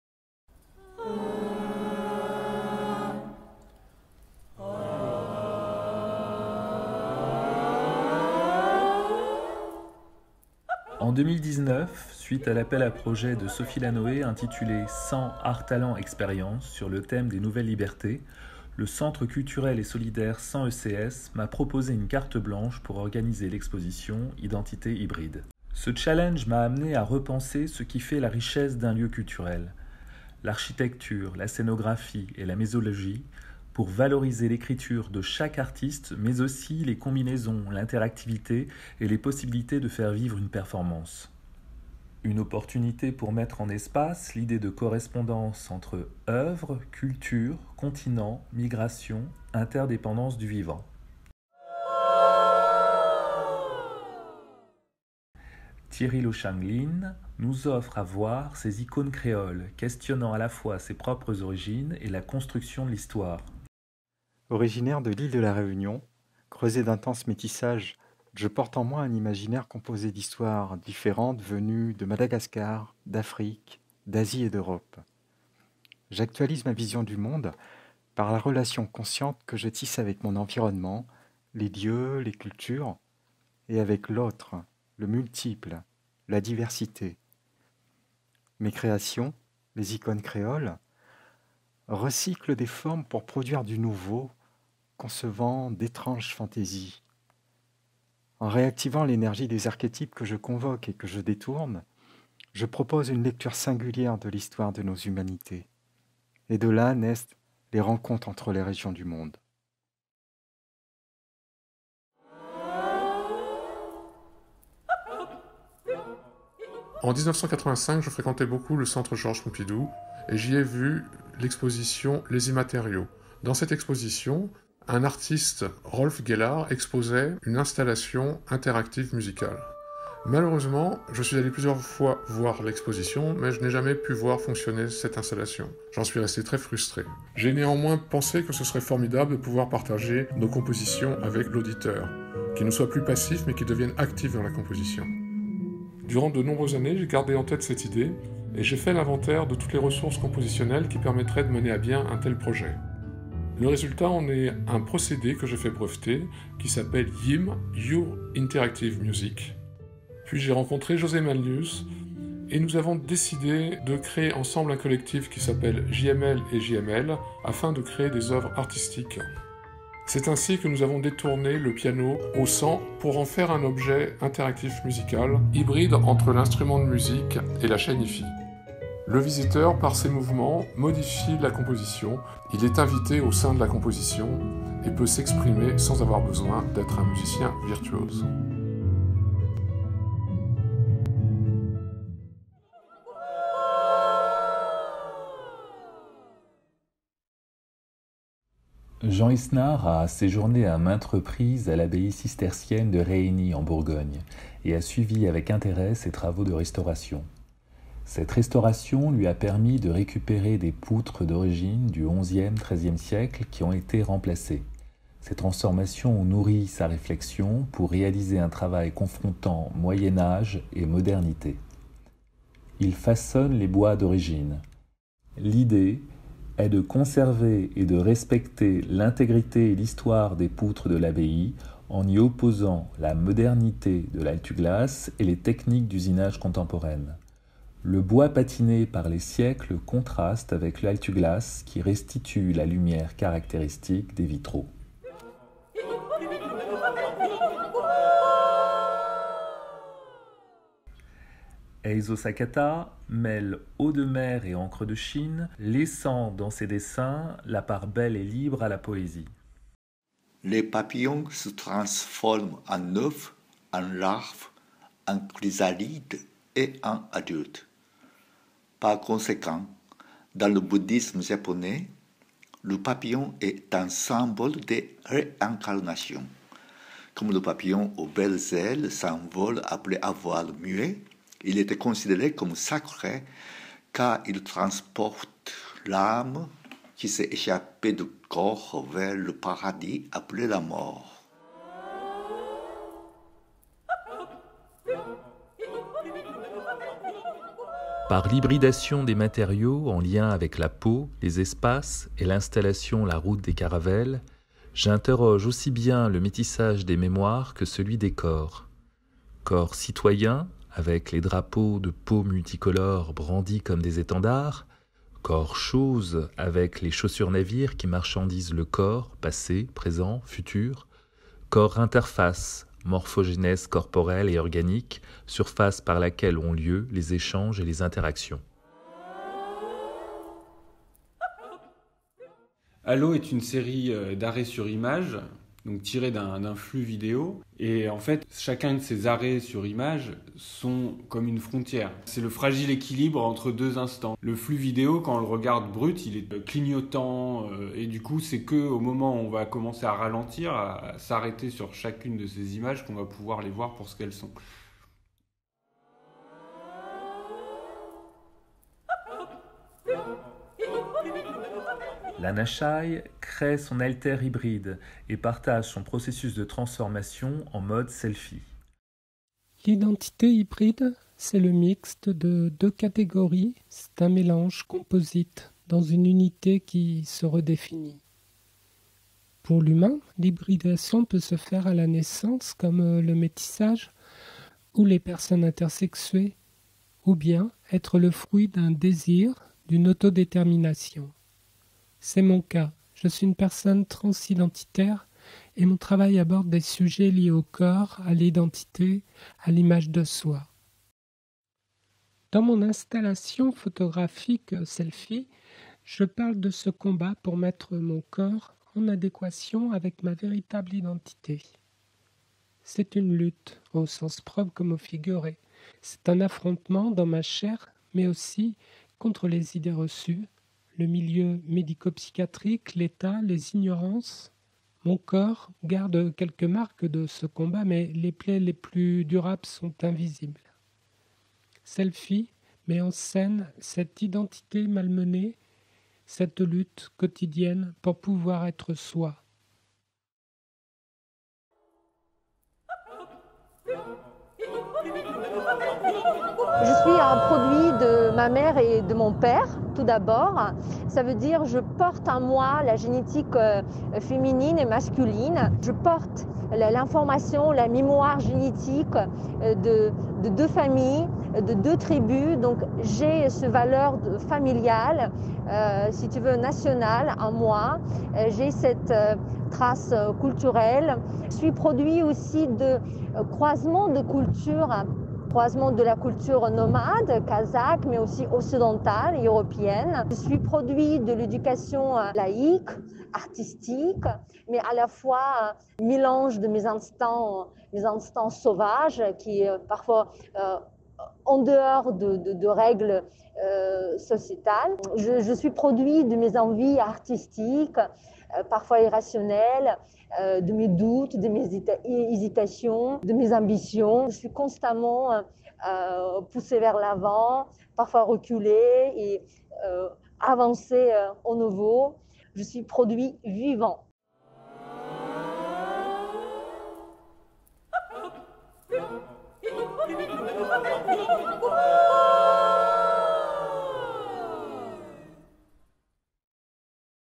Exposition IDENTITÉS HYBRIDES
Le 100ecs présente IDENTITÉS HYBRIDES. Cette exposition collective de neuf artistes, sous la houlette de José Man Lius, croise des regards multiples, tente de libérer chimères et mémoires, et de rendre présent l’inimaginable. Elle nous invite ainsi à un espace tout à la fois singulier et pluriel loin des catégorisations habituelles, entre étendues des possibles et concentré du Tout-monde cher à Edouard Glissant.

Une proposition de José Man Lius avec la participation de :
Dana Mussa, Danseuse, chorégraphe
Eizo Sakata, Plasticien
Jean Isnard, Sculpteur
Jean-Marie Lavallée, Compositeur, musicien
José Man Lius, Plasticien
JML & JML, Collectif mixte média
Konpyuta, Vidéaste
Lanah Shaï, Performeuse multimédia
Thierry Lo Shung Line, Plasticien

- Exposition du 1er au 30 avril 2021
- Contactez l’équipe du 100ecs pour les visites professionnelles !
- T. Roxanne & Isabelle | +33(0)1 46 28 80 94
- 100ecs | 100, rue de Charenton, 75012 Paris

L’exposition IDENTITES HYBRIDES est l’aboutissement d’un parcours, qui nous conduit un an après le premier confinement à rendre hommage à la ténacité de son commissaire, José Man Lius, et des artistes participants, dont le talent collectif a résisté aux annulations et contraintes successives, ainsi qu’à la morosité ambiante. Hommage également à l’établissement culturel et solidaire du 100, qui a permis un entre-deux afin que cet événement se tienne en mars 2021 dans un espace qui lui correspond.

Alors qu’une chimère d’un genre inédit traverse nos territoires, IDENTITES HYBRIDES nous convie à un voyage pour percevoir le brassage des identités et des altérités, comme autant de voies d’ouverture vers un imaginaire décloisonné.
En libérant le champ des possibles, que ce soit par l’hybridation des matériaux et des techniques, par celle des formes d’expression et des mémoires, ou encore par la métamorphose en autoportrait, cette exposition nous place résolument dans une perspective d'Art global transdisciplinaire.

Ce regard innovant nous permet de mieux appréhender le monde de demain, alors que nous nous tenons au carrefour de multiples transmutations à venir.
Par le jeu de correspondances des œuvres, c’est également la créolisation qui opère, loin des catégorisations habituelles, entre étendues imaginaires et concentré du Tout-monde. En mêlant Art, poésie et énigme, IDENTITES HYBRIDES nous propose ainsi une part de rêve qu’il convient de savourer car, comme l’écrivait Albert Einstein, « l'expérience la plus belle et la plus profonde que puisse faire l'homme est celle du mystère. ».
Barbara Marshall, auteure

Ce projet a été imaginé par José Man Lius, curateur en collaboration avec l'équipe du centre culturel  du 100ecs et CULTURAMA. Remerciements à Barbara Marshall, auteure, Grigoriy Manucharian, traducteur, Culturama & Résonance graphique, Design & graphisme. Bande son. Marie-Sylviane Buzin.